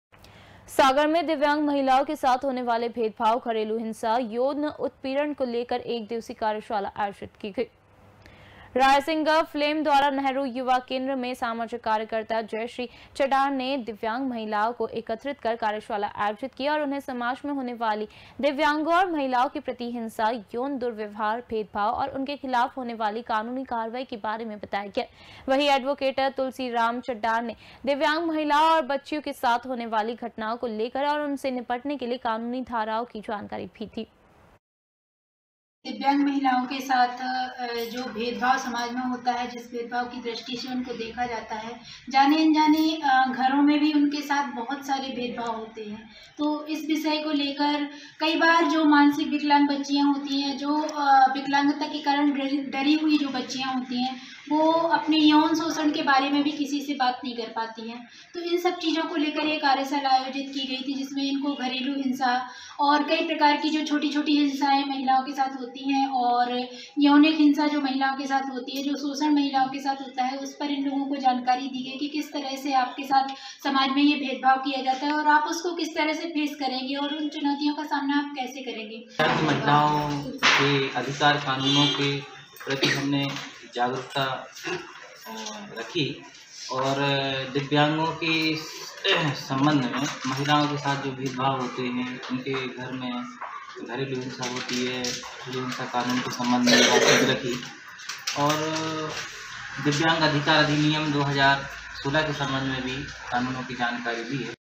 भेदभाव, घरेलू हिंसा को लेकर कार्यशाला आयोजित, महिलाओं को कानूनी कार्रवाई की जानकारी दी...
भेदभाव, घरेलू हिंसा को लेकर कार्यशाला आयोजित,
महिलाओं को कानूनी कार्रवाई की जानकारी दी...

BREAKING NEWS